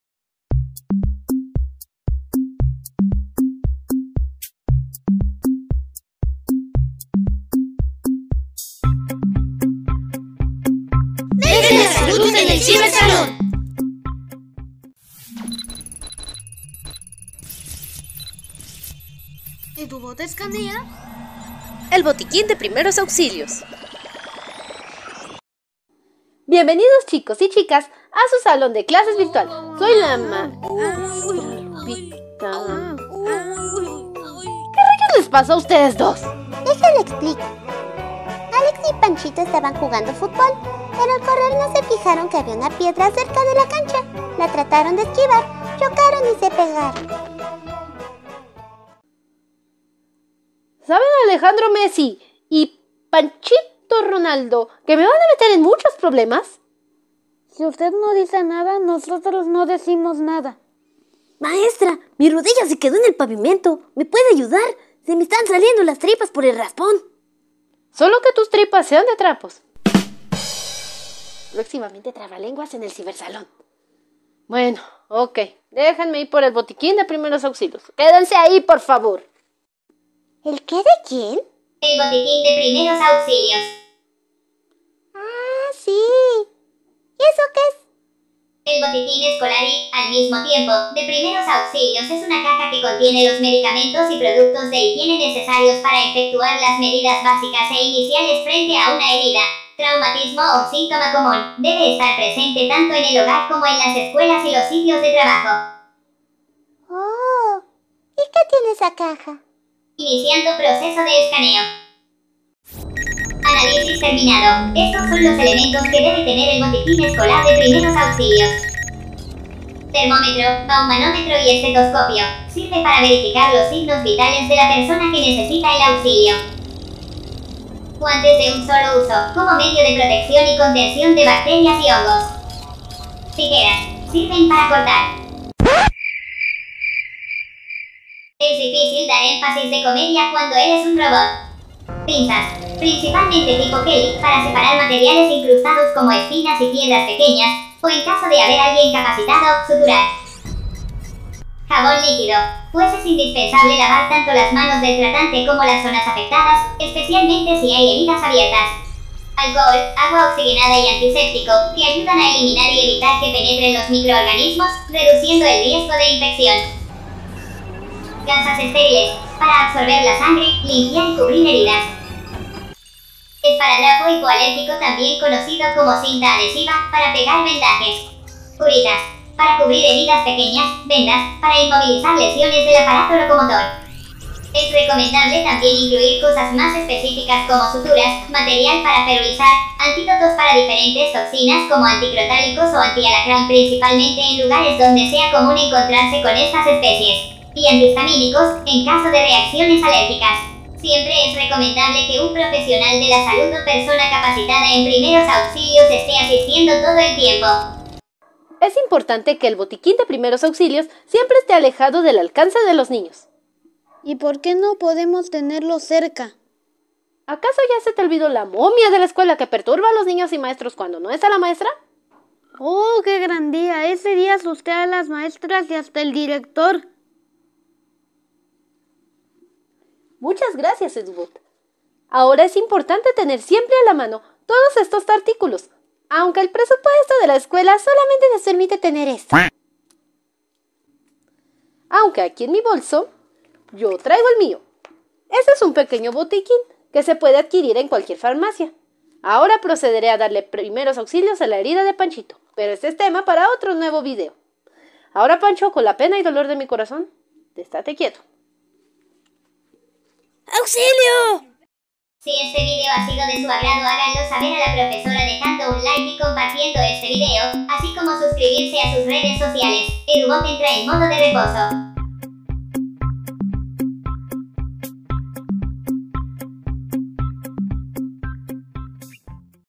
Edubot escanea el botiquín de primeros auxilios MesDeLaSalud (Arma tu propio botiquín)
¿sabes que hacer si ocurre algún accidente en tu salón de clases? hoy edubot nos muestra los elementos mas importantes de el botiquín de primeros auxilios

Do you know what to do if an accident occurs in your classroom? today edubot shows us the most important elements of the first aid kit